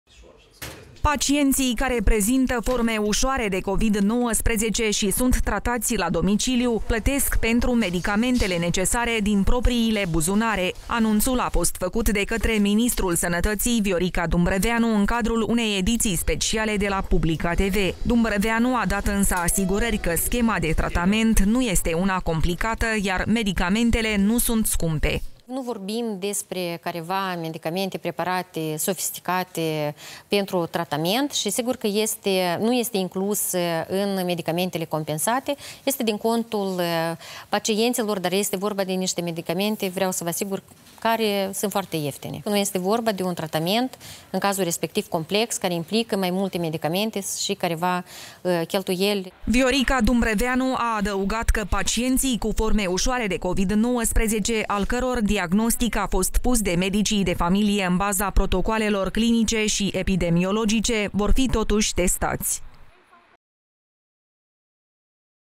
Viorica Dumbrăveanu: Pacienții cu COVID-19, care sunt tratați la domiciliu
Viorica Dumbrăveanu: Pacienții cu COVID-19, care sunt tratați la domiciliu, plătesc pentru medicamentele necesare din propriile buzunare